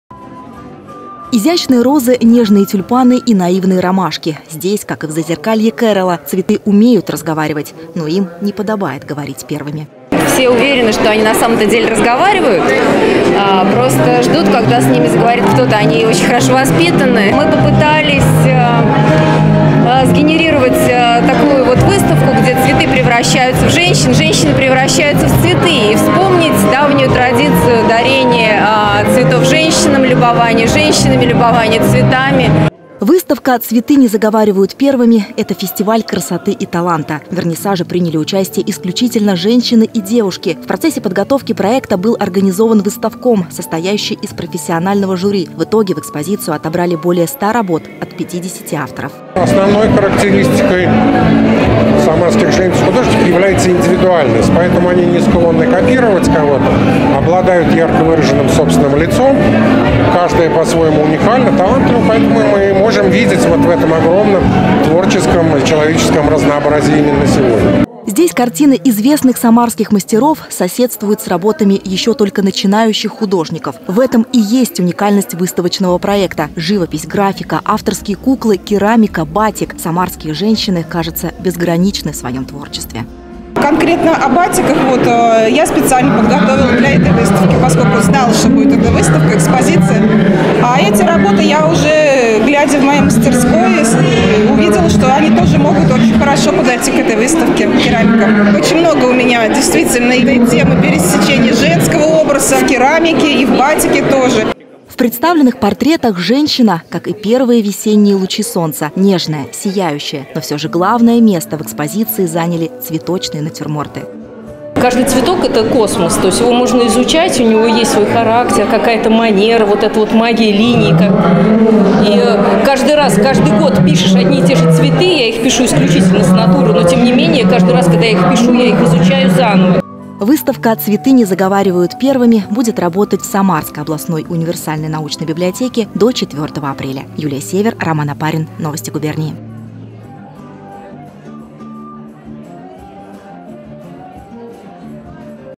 В преддверии 8 Марта в Самаре открылась выставка "Цветы не заговаривают первыми"
В Центре культуры и творчества "Новое пространство" Самарской областной универсальной научной библиотеки открылась выставка "Цветы не заговаривают первыми". В экспозицию вошли более ста работ от 50 авторов. Участниками экспозиции стали исключительно девушки.